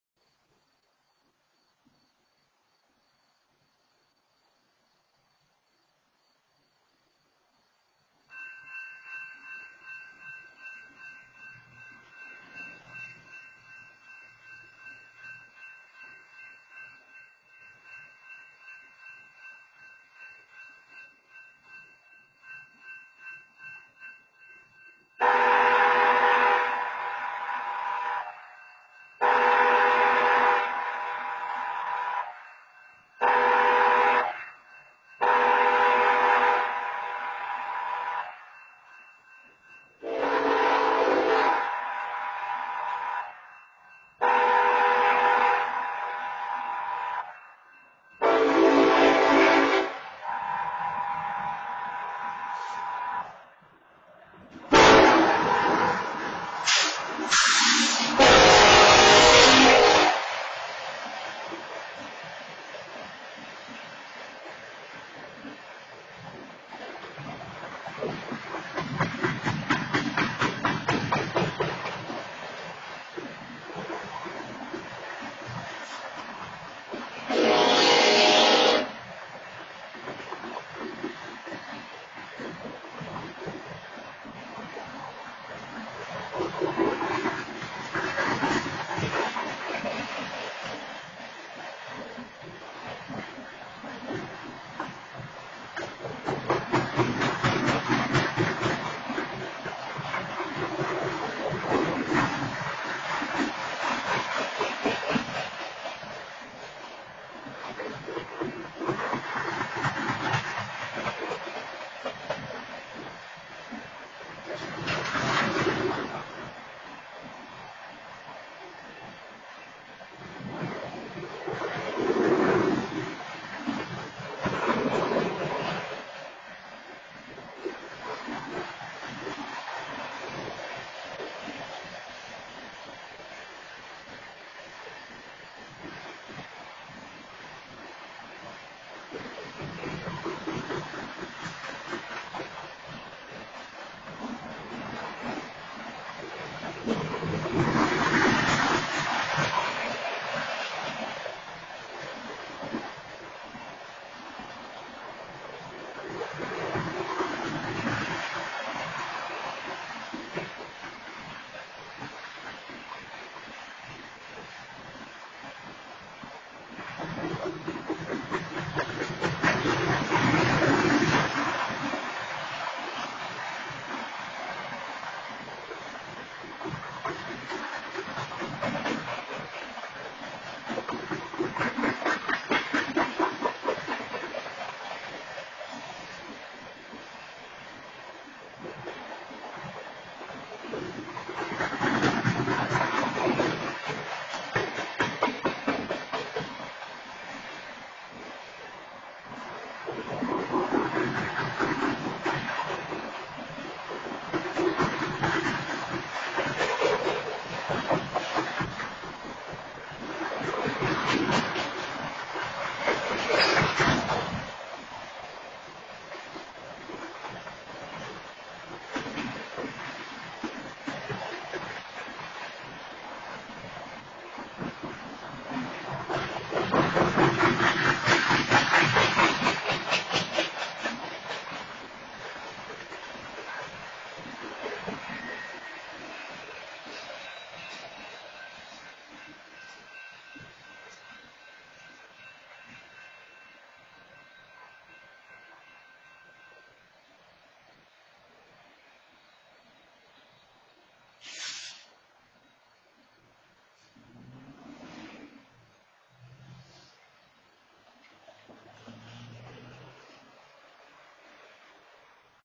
20180520 2359 NB BNSF Engine honks LLSL at active wayside crossing 67th street and honks again at 65
At 23:59pm on 5/20/2018, a train sounded its on-board horn at 663559T, an actively sounding wayside horn crossing in Merriam, Kansas.
- The train was headed Northbound at 12:01:00 AM
- The engine was painted in BNSF's color scheme/pattern
- The orange blinking X indicators were blinking and visible to train driver on approach to the crossing.
- The waysides produced adequate sound to hear them from a distance.
- No obstructions were present at the crossing.
- The Honk pattern was: LONG LONG short LONG (This demonstrates that the honking was not warning an animal or person on the tracks, but was intended to announce its intention to cross a road.)
- The DOT number of the crossing is: 663559T
- The precipitation at the time was: None
- The road conditions were: Wet
Other notes from this event:
This train also honked at 65th/Carter - 663558L (another wayside crossing). This is audible in the public video.

This video and incident report was provided to Andy Williams in BNSF Public Affairs on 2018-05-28.